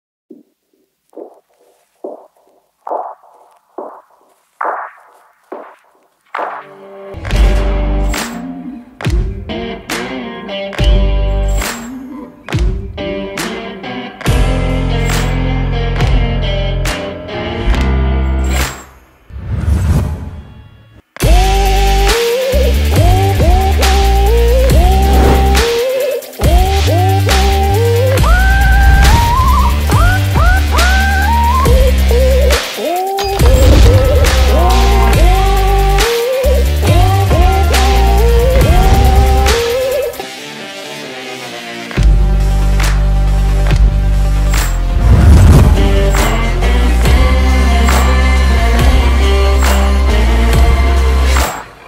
LAPTOP B ROLL CHALLENGE | HAND HELD CAMERA MOVES | CINEMATIC MONOCHROME VIDEO USING SONY A7
i shot this video at home during lockdown .
Gear Used- SONY A7|||

Help me with your valuable suggestions and ideas.